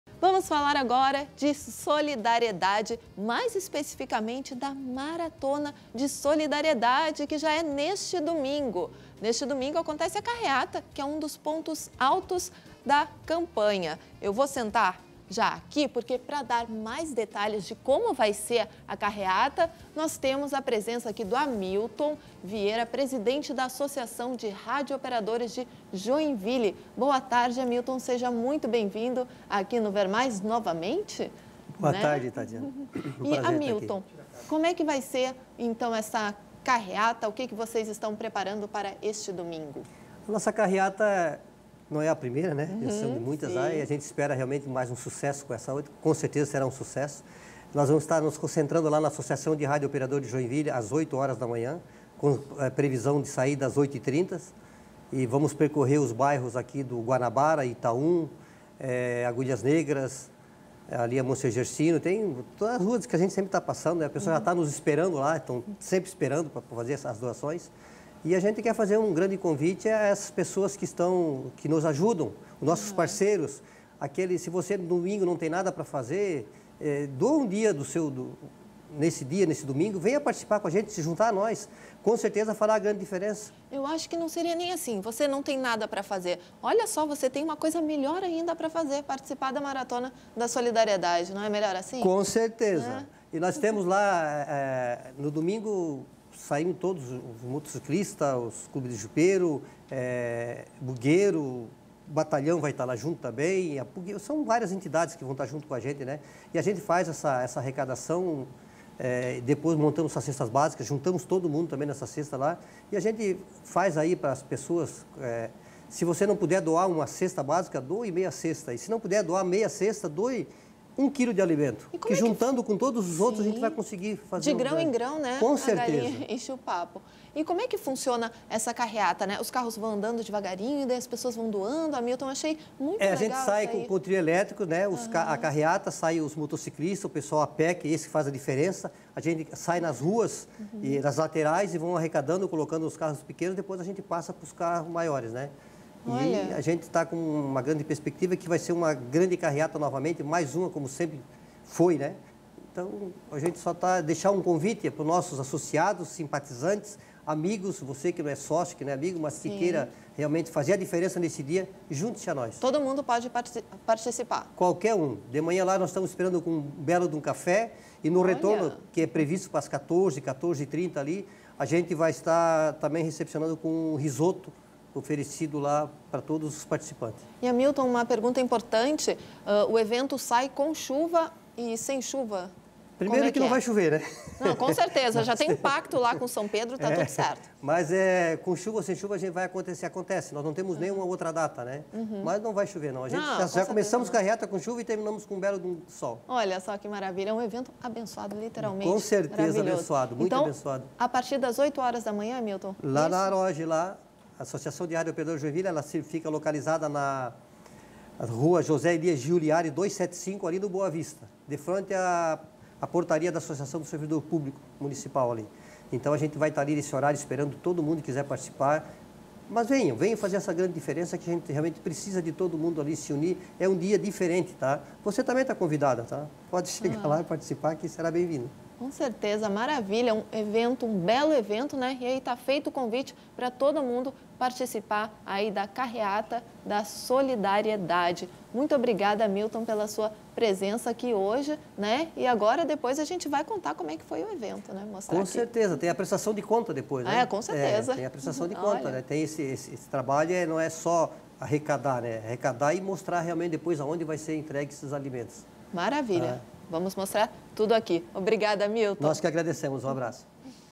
Carreata da Maratona da Solidariedade acontece neste domingo em Joinville
Neste domingo (24) acontece a Carreata da Maratona da Solidariedade, que é um dos pontos altos da campanha. Pra dar mas detalhes de como vai ser, um dos organizadores, o Hamilton Vieira, presidente da Associação de Rádio Operadores de Joinville, esteve no estúdio do Ver Mais.